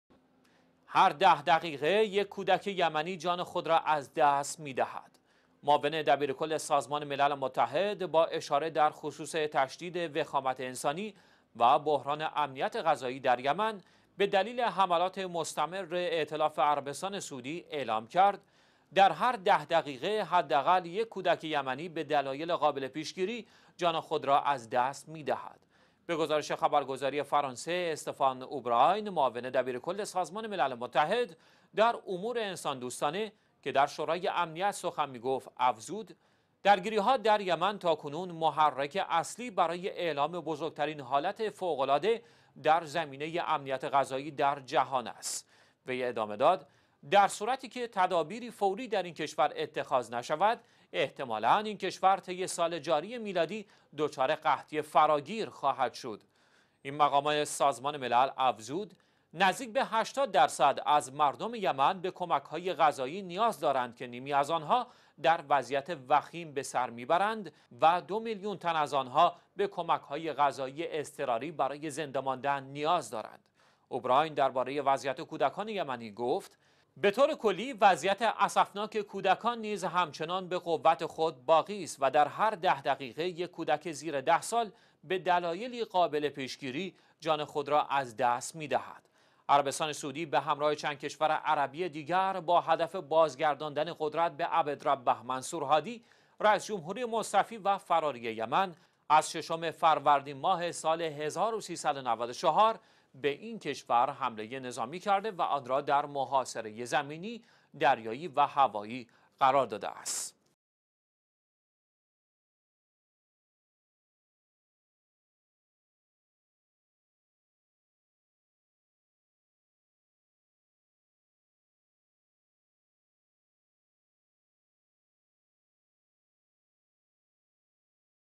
کودکان یمنی، بزرگ ترین قربانیان حملات ائتلاف سعودی به یمن / ۹ بهمن ۱۳۹۵
کودکان یمنی، بزرگ ترین قربانیان حملات ائتلاف سعودی به یمن

در هر 10 دقیقه یک کودک یمنی، جان خود را از دست می دهد.

معاون دبیرکل سازمان_ملل متحد با هشدارِ تشدید وخامت انسانی و بحران امنیت غذایی در یمن به‌دلیل حملات مستمر ائتلاف عربستان سعودی، اعلام کرد: در هر 10 دقیقه ،حداقل یک کودک یمنی به دلایل قابل پیشگیری جان خود را از دست می‌دهد.

به گزارش خبرگزاری فرانسه «استفان اوبراین» معاون دبیرکل سازمان ملل متحد در امور انسان دوستانه، که در شورای امنیت سخن می گفت، افزود: درگیری‌ها در یمن تاکنون محرک اصلی برای اعلام بزرگ‌ترین حالت فوق‌العاده در زمینه امنیت غذایی در جهان است.

وی ادامه داد: در صورتی‌که تدابیری فوری در این کشور اتخاذ نشود، احتمالا این کشور طی سال جاری میلادی دچار قحطی فراگیر خواهد شد.

این مقام سازمان ملل افزود: نزدیک به 80 درصد از مردم یمن به کمک‌های غذایی نیاز دارند که نیمی از آنها در وضعیت وخیم به سر برده و دو میلیون تن از آنها به کمک‌های غذایی اضطراری برای زنده ماندن، نیاز دارند.

اوبراین در باره وضعیت کودکان یمنی گفت: به طور کلی، وضعیت اسفناک کودکان نیز همچنان به قوت خود باقی است و در هر 10 دقیقه یک کودک زیر 10 سال به دلایلی قابل پیشگیری، جان خود را از دست می‌دهد.

عربستان سعودی به همراه با چند کشور عربی دیگر، با هدف بازگرداندن قدرت به «عبدربه منصور هادی» رئیس‌جمهوری مستعفی و فراری یمن، از 6فرودین ماه سال 1394  به این کشور حمله نظامی نموده و آن را در محاصره زمینی، دریایی و هوایی قرار داده است.

شنبه - ۹ بهمن ۱۳۹۵
السبت - ٢٩ ربيع الثاني ١٤٣٨